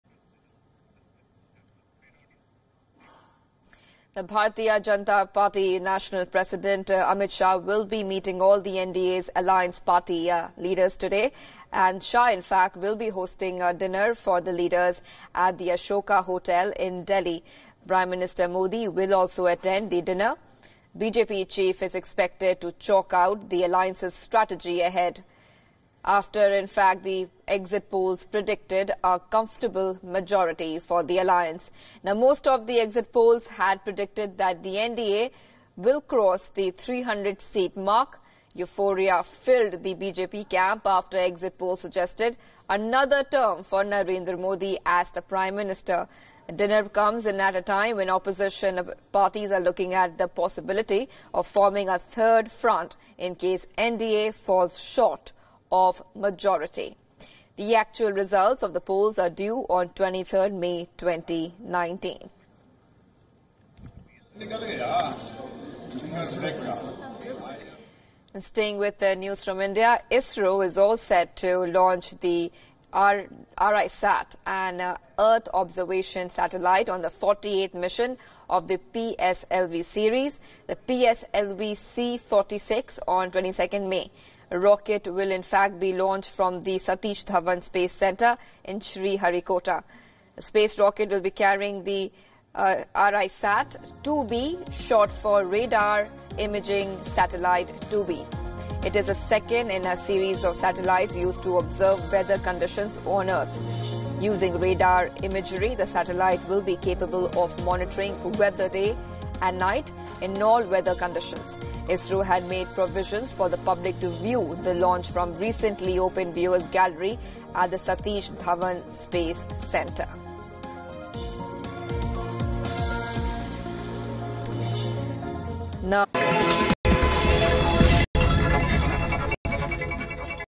Amit Shah to meet allies, host dinner on Tuesday ahead of 23rd May results
With exit polls forecasting NDA's return to power, top leaders of the BJP-led alliance will meet on Tuesday over dinner to be hosted by party president Amit Shah where Prime Minister Narendra Modi is expected to be present, party sources said on Monday.